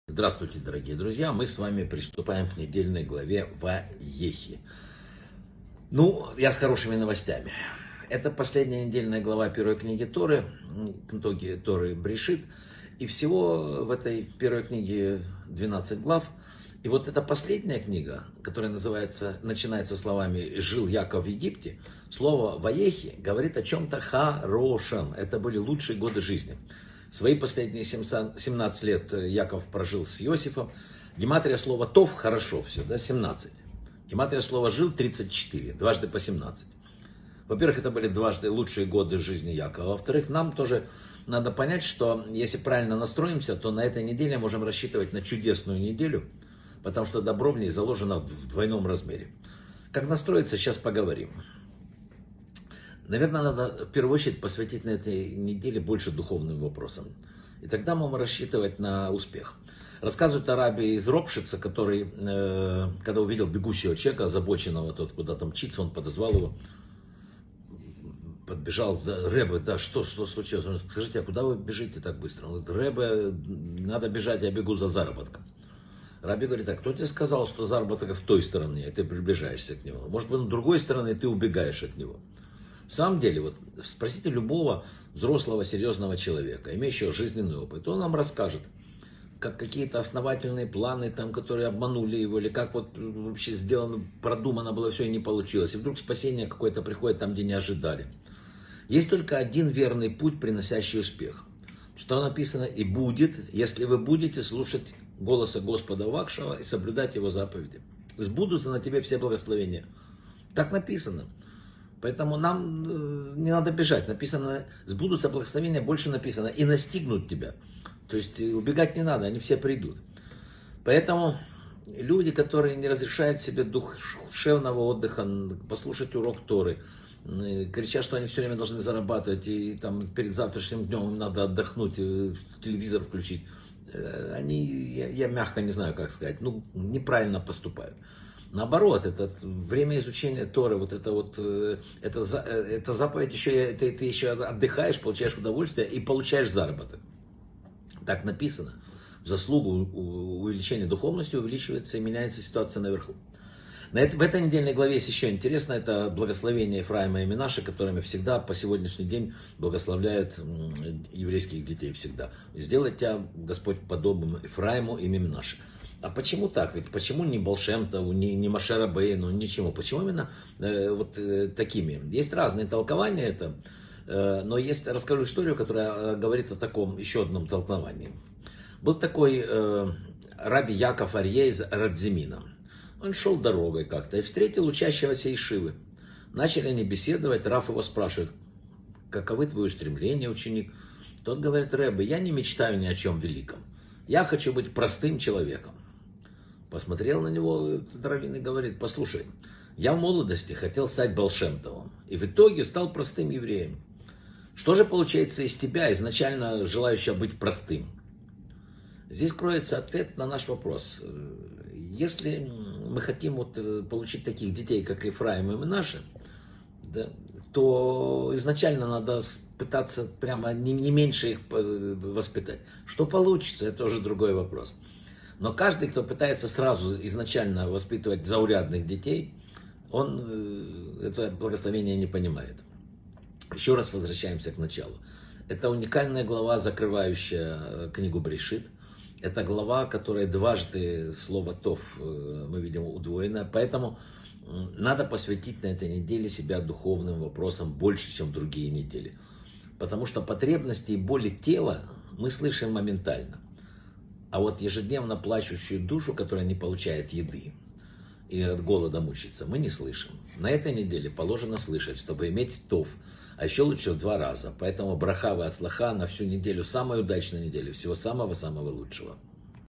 📘 Духовность - единственный путь к успеху. Недельная глава Ваехи | Вадим Рабинович
Какой намек есть в названии последней главы Книги Берейшит?
Почему именно сейчас особенно важно обратить внимание на духовность?
Какой вопрос задал раби из Ропшица бегущему человеку?
Почему не стоит бежать за благословениями и благодаря чему они придут к нам сами?
Каким образом исполнение заповеди изучения Торы может стать не только полезным отдыхом, но и привести к заработку?
Почему, когда мы благословляем сыновей, мы желаем им быть "как Эфраим и как Менаше"? История о раби Яакове из Радзимина и юноше, изучающем Тору.
Потребности тела мы слышим всегда, а что с духовностью? Плач нашей души.